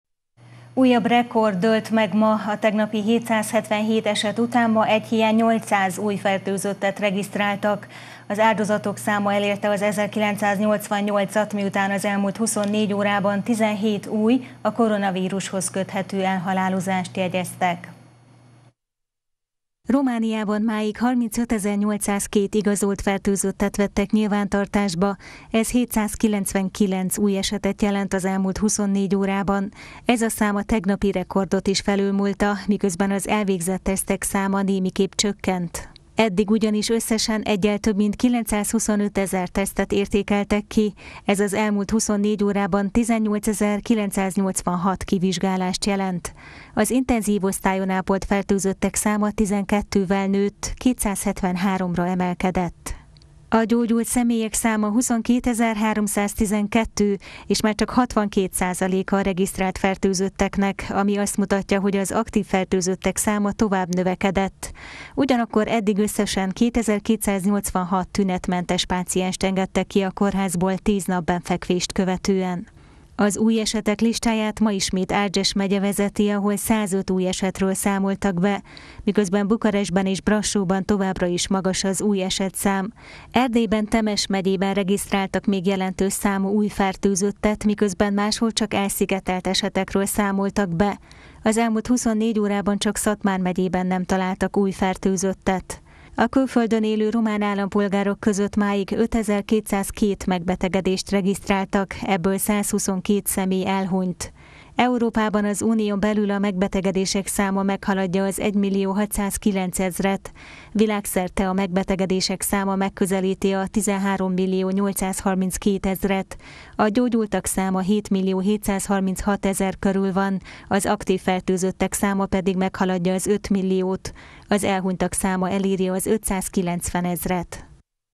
Közel 800 új eset – Erdélyi Magyar Televízió
Újabb rekord dőlt meg ma: a tegnapi 777 eset után ma 799 új fertőzöttet regisztráltak. Az áldozatok száma elérte a 1988-at, miután az elmúlt 24 órában 17 új, koronavírushoz köthető elhalálozást jegyeztek.